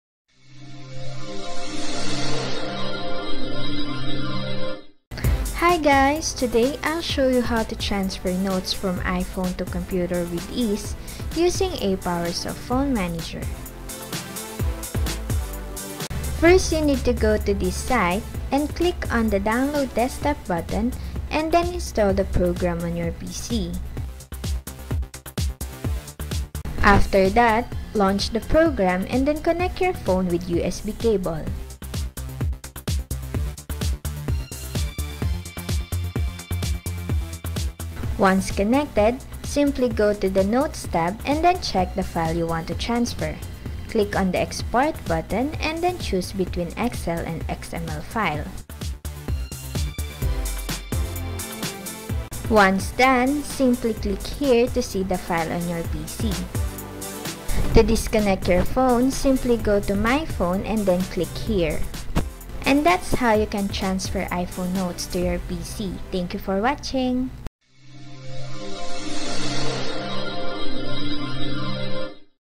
How to Transfer Notes from iPhone to Computer with Ease
This video shows you how to transfer notes from iPhone to PC easily which may help you save some important notes.